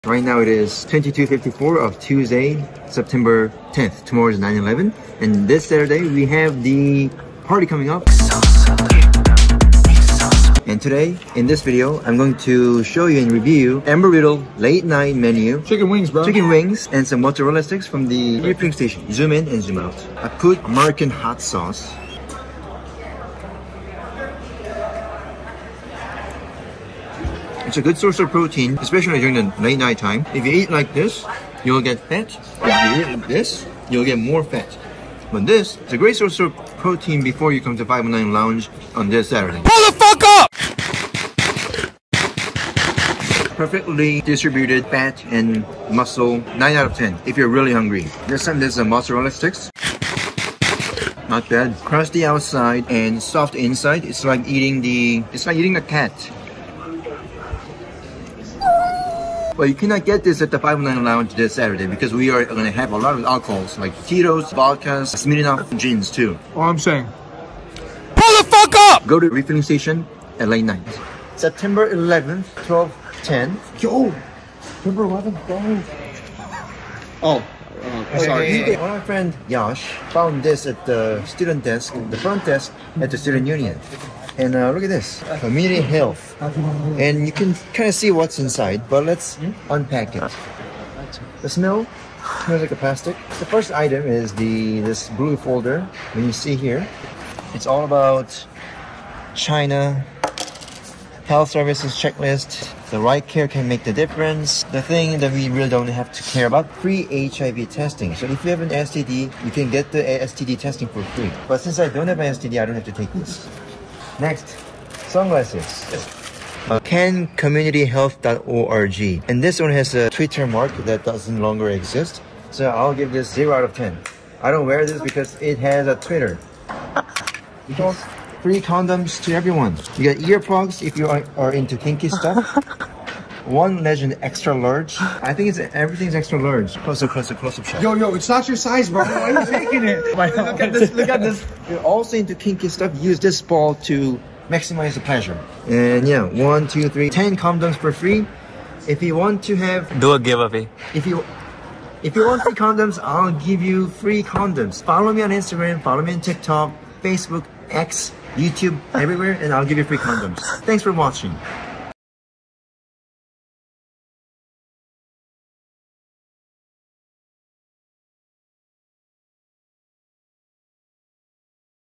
I TRIED CHICKEN WINGS FROM MY UNIVERSITY !!!
Hello my name is Brian Park, I am the master of this channel. In this video, I tried Embry Riddle Aeronautical University's late night chicken wings and mozzarella sticks. I think chicken wings were more healthier.

⚙️ Gears I used for the video 👇
📍Computer: MacBook Pro 16 inch 2021 M1 Pro
📍Camera: iPhone 15 Pro
📍Editing Software: Final Cut Pro X